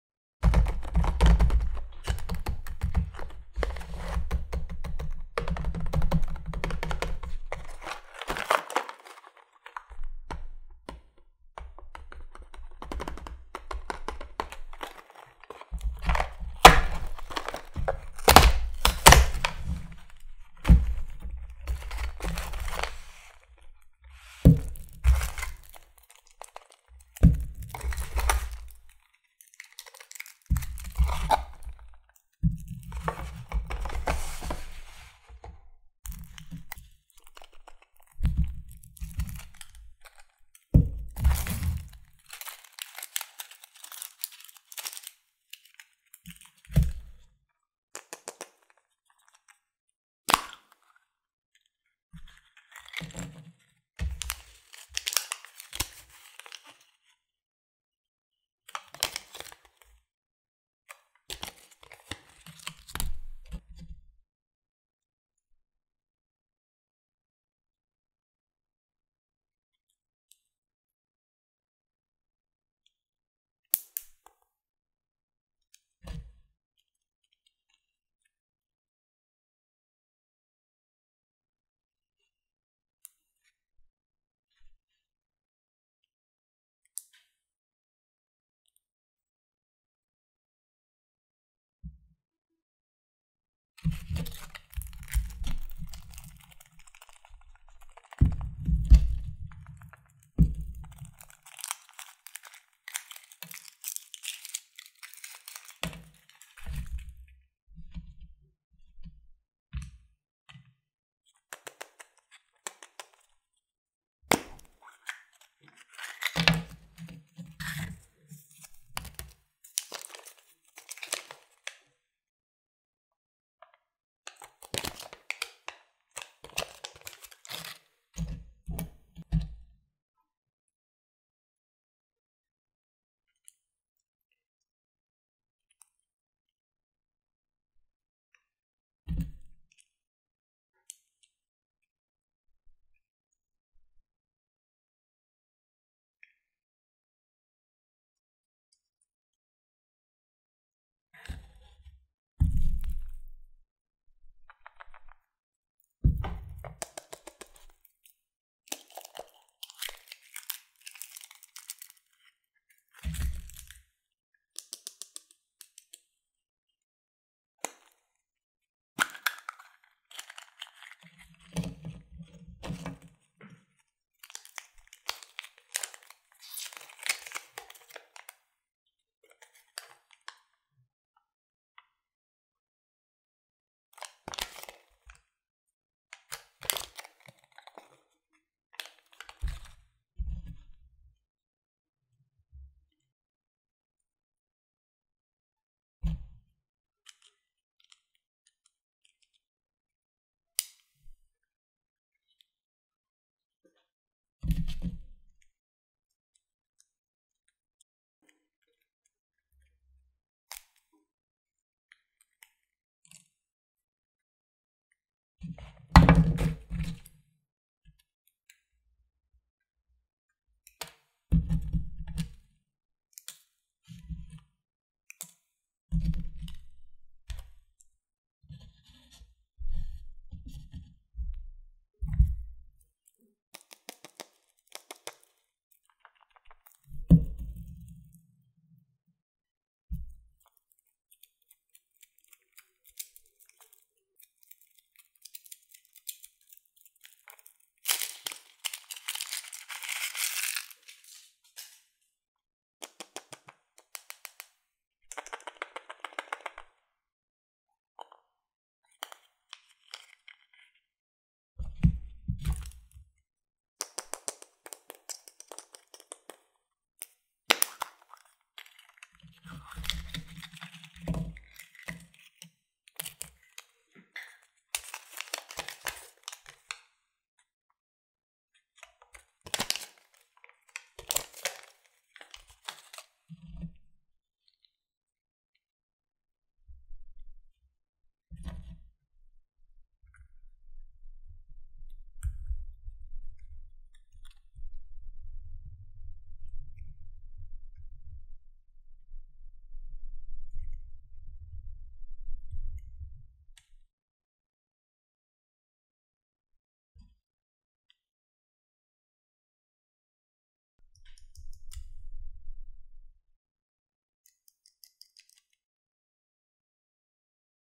NEW!!! Frozen Edition Kinder Surprise Box with 4 Eggs l Exciting ASMR Unboxing from Germany
❄️✨ Immerse yourself in the enchanting world of Frozen with our ASMR unboxing of the Kinder Surprise Box featuring 4 eggs in the Frozen Edition, straight from Germany! Join us as we unwrap the magic of Elsa, Anna, and Olaf, with each egg containing delightful surprises inspired by the beloved characters. The soothing sounds of crinkling wrapping paper, gentle tapping, and the joy of discovering what's inside make this unboxing a magical ASMR experience. Let the Frozen Edition Kinder Surprise eggs transport you to a winter wonderland of sweet surprises! 🍫❄️